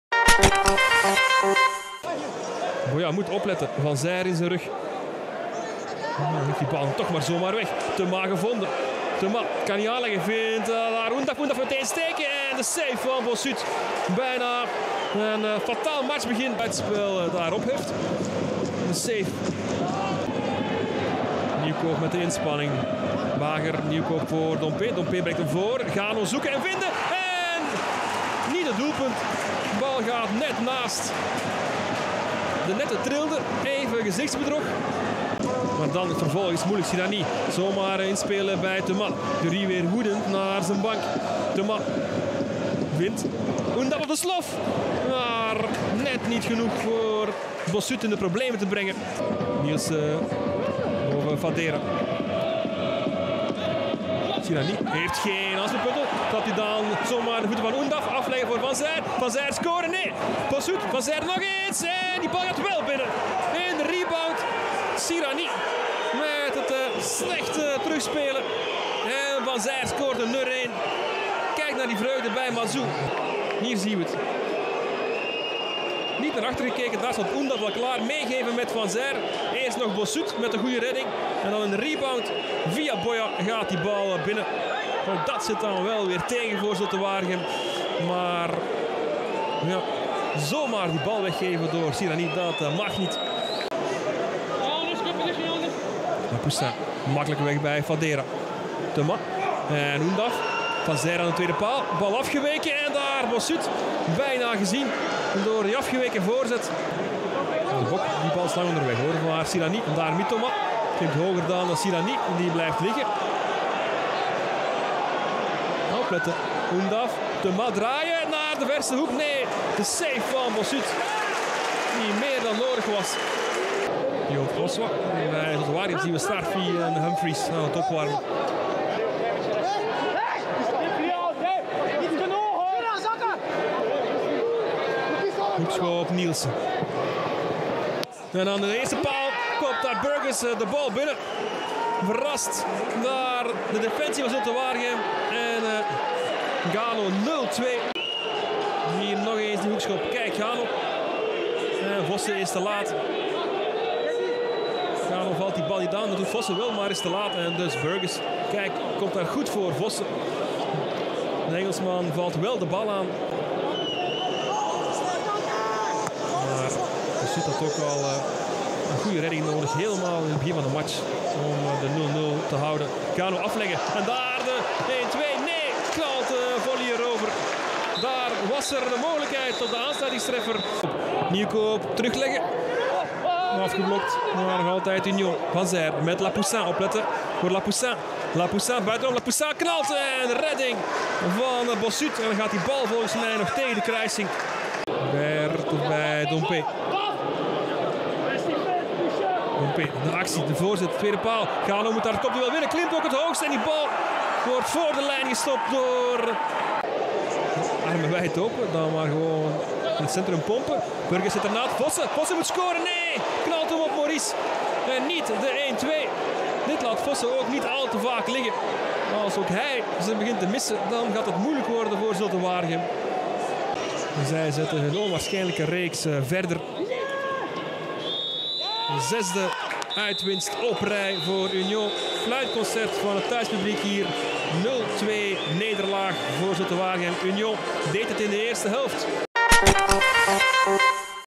S21-22 | Speeldag 19: Essevee - Union SG
Essevee ontving leider Union in de Elindus Arena. De wedstrijd eindigde op 0-2, bekijk hier de samenvatting.